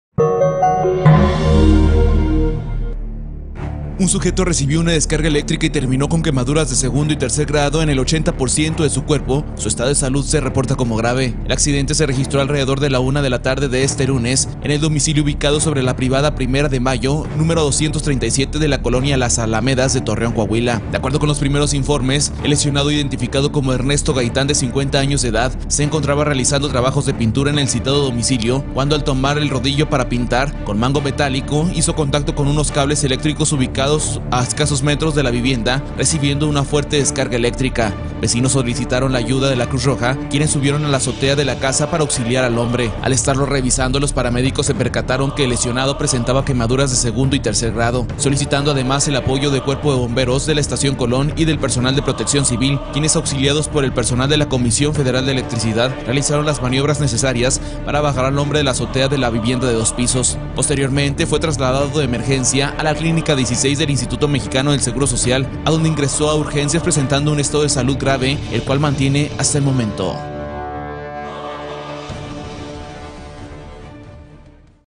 Sufre descarga eléctrica y lo deja con 80% del cuerpo quemado
Realizaba trabajos de pintura en una casa habitación en Torreón.